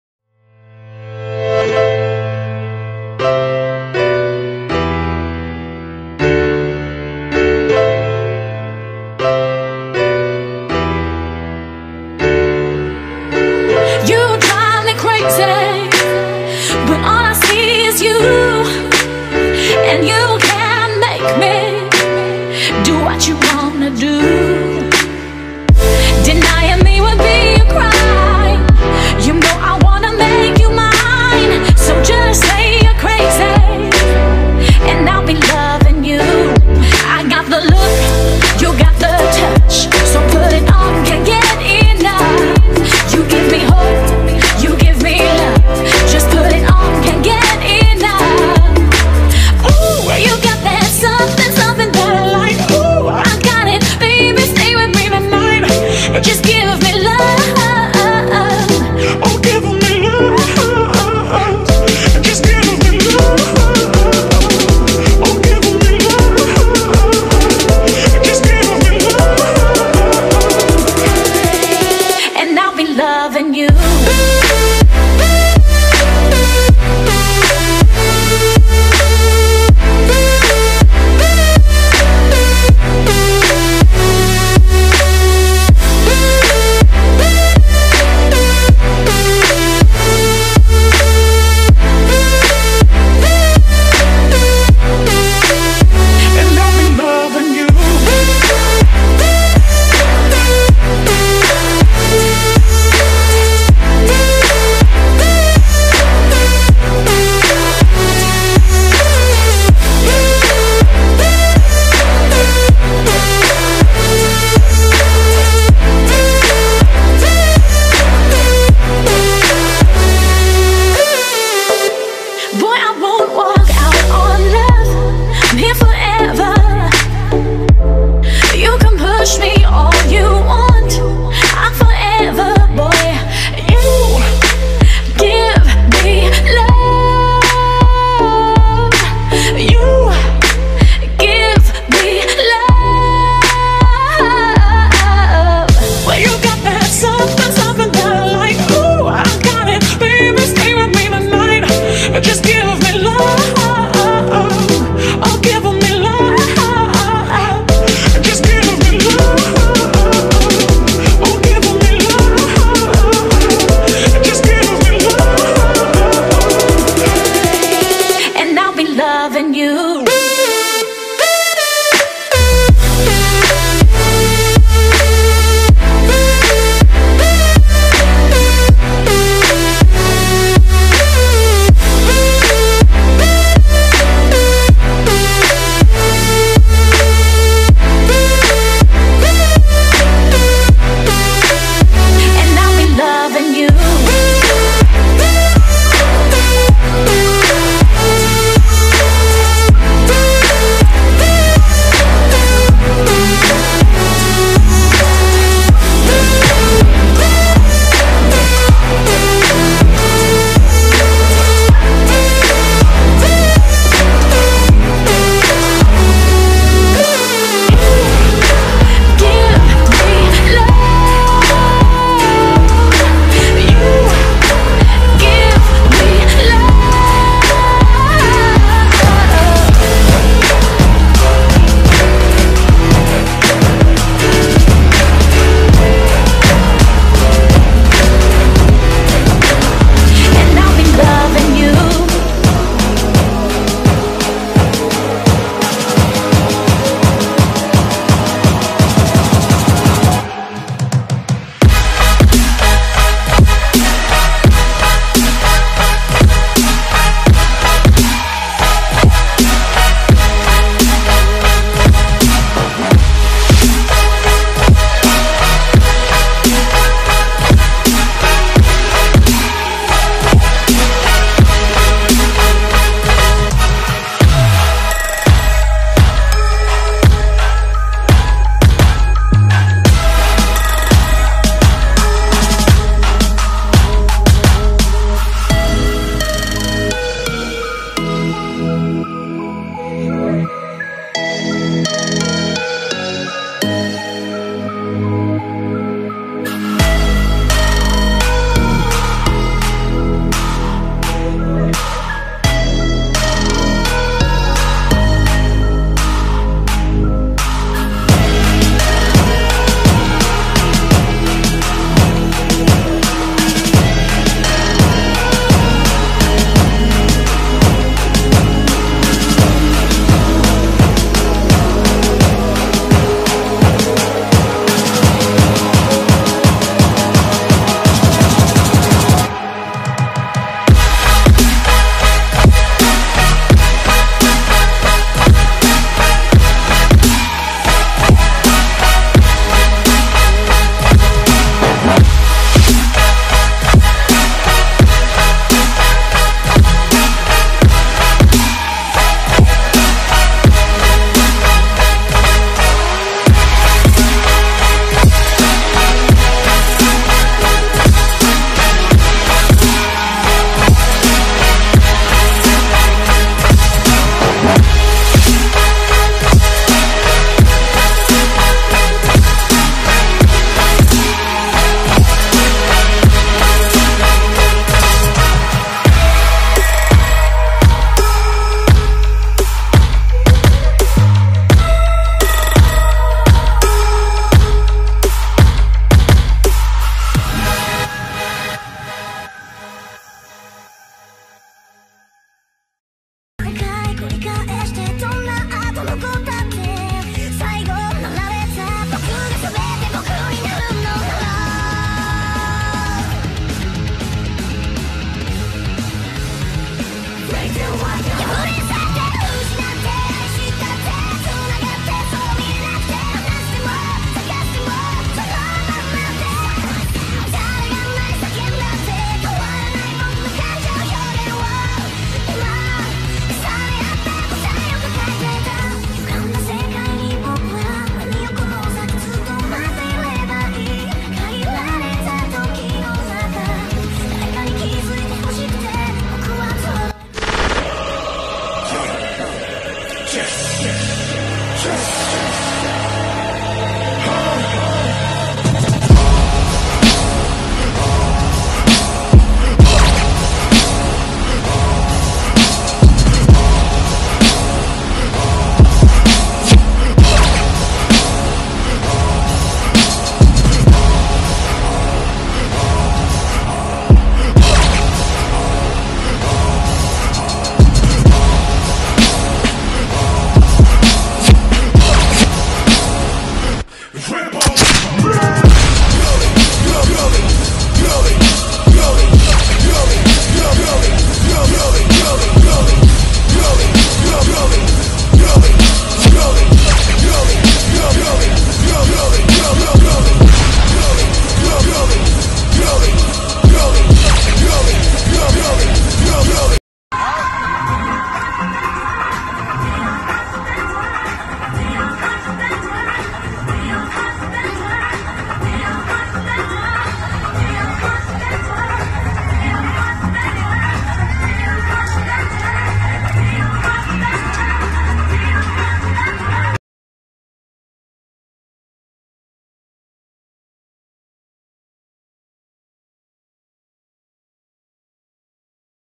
FULL PLANCHE GODS CALISTHENICS | STREET WORKOUT 2019 MOTIVATION
Some of the best humans doing the full planche and variations